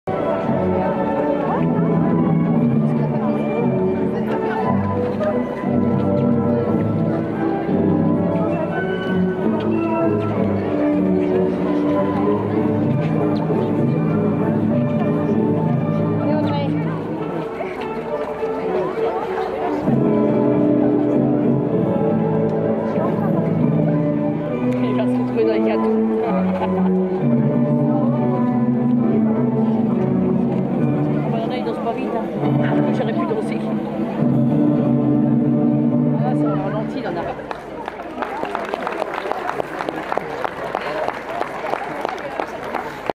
.Au temps de la crinoline - Nuit des Musées - Hôtel de Ville d’Étampes - Valse
Au temps de la Crinoline vous propose un spectacle original et élégant afin de rendre hommage à l’art vivant et à l’histoire. Ce groupe composé de danseurs et danseuses vous invitera au voyage à un inoubliable voyage dans le temps. En costumes et en robes d’époque, ils danseront les quadrilles des lanciers, russes, américains, ainsi que la valse, la polka et tant d’autres encore.